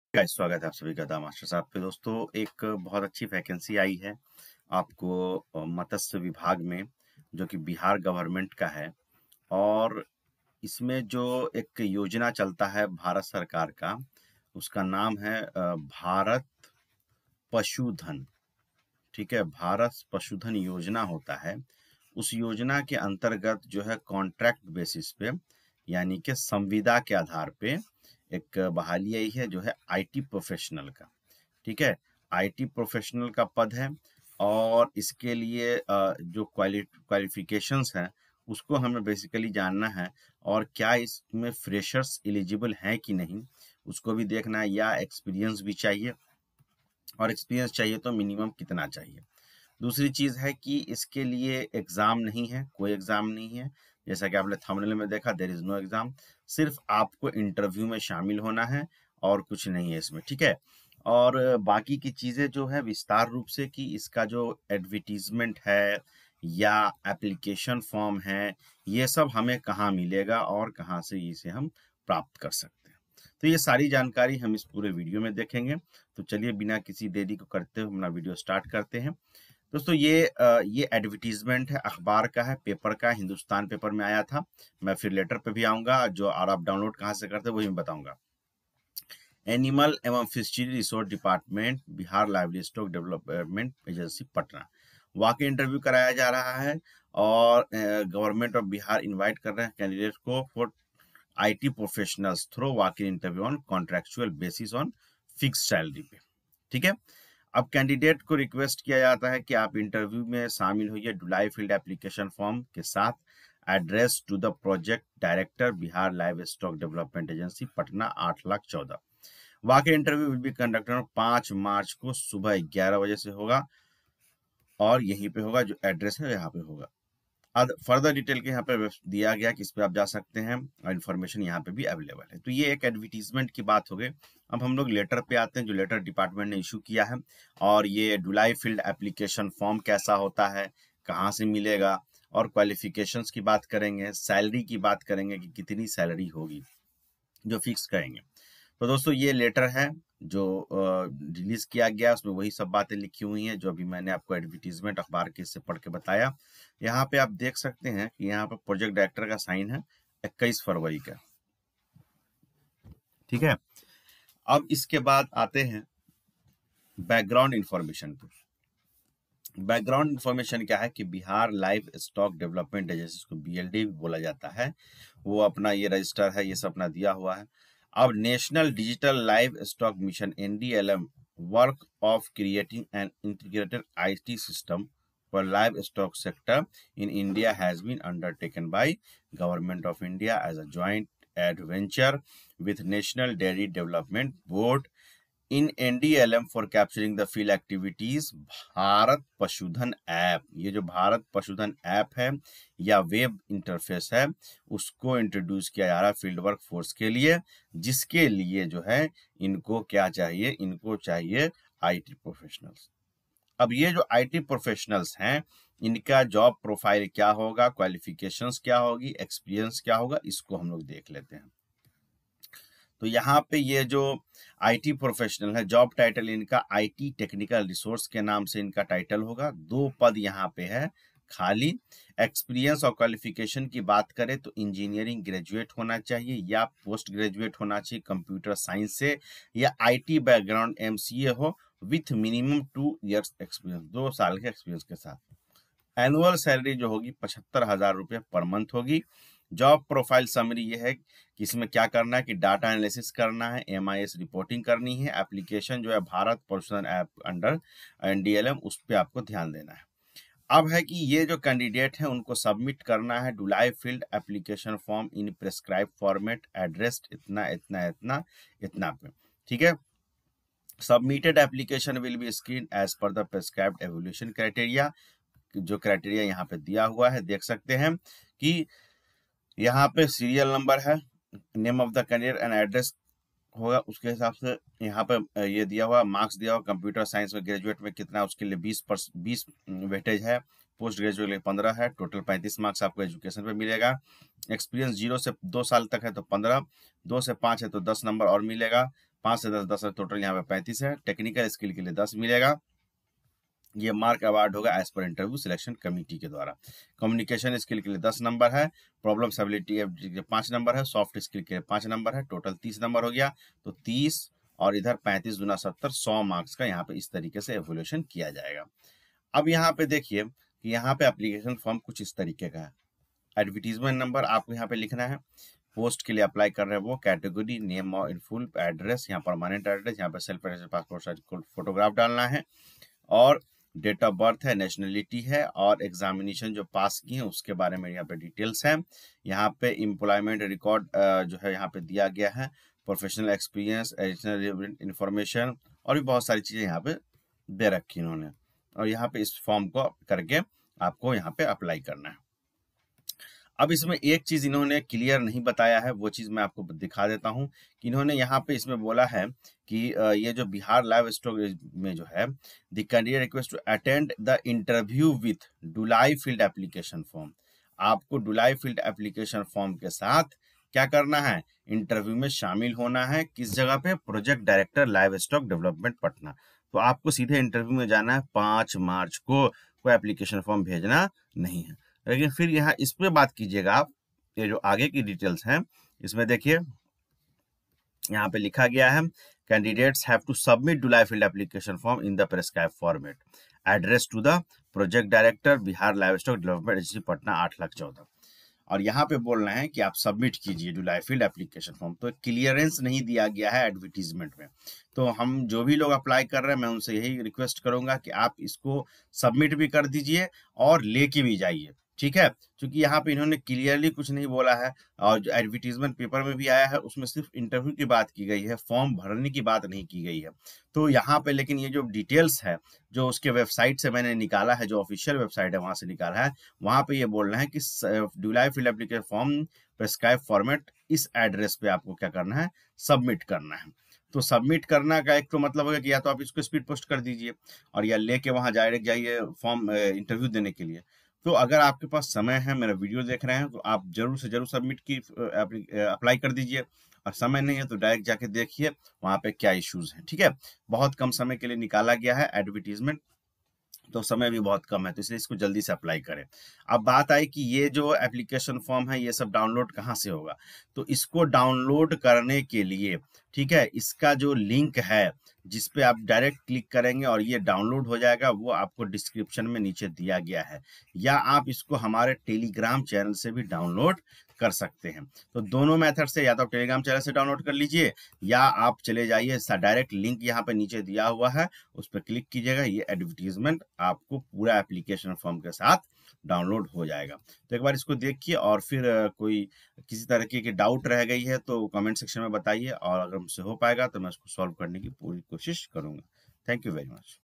Bihar gov new vacancy 2024 | IT professionals | Contract based | No exams
Bihar gov new vacancy 2024
Post - IT professionals

Interview time - 11 AM